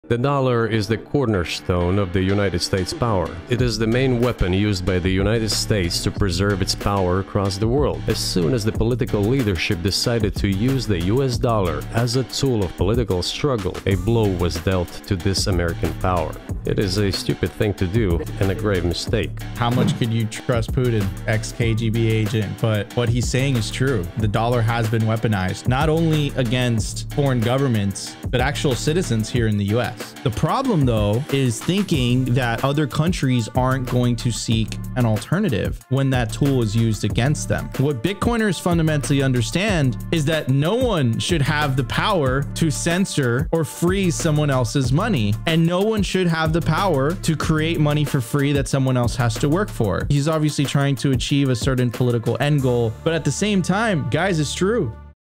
The US Dollar as a Political Tool EXPOSED!!
The interview that almost broke the internet exposes the political tool of printing money and its detrimental effects.